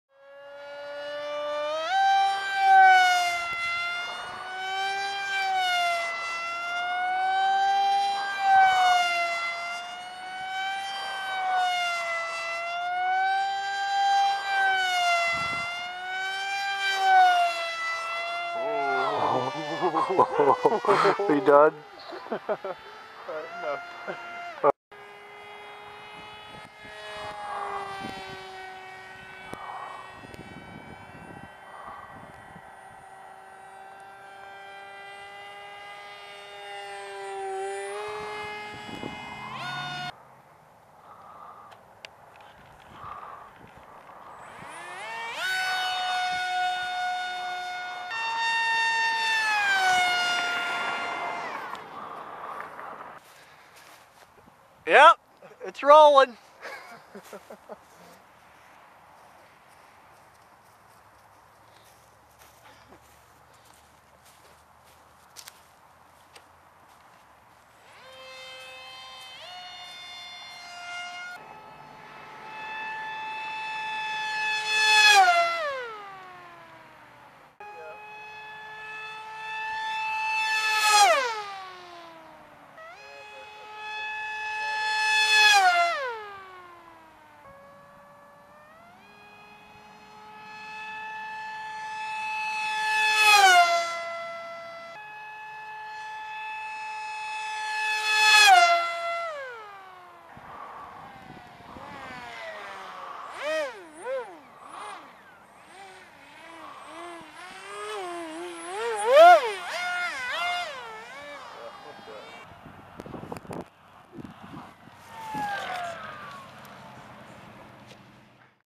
HobbyKing - SkyFun ARF Pusher Jet 100 + mph radar gun
fast fast park jet 100 + mph !!! speed testing with radar gun ! HobbyKing - SkyFun ARF Pusher Jet

S